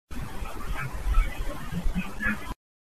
Early afternoon EVP
The bedroom camera recorded the event apparently reacting to what sounds like a conversation.
Its for the most part inaudible but the last word sounds to me like its saying 'schedule'.
Its been pretty quiet here for the past couple months so, nice to know they're still hanging around.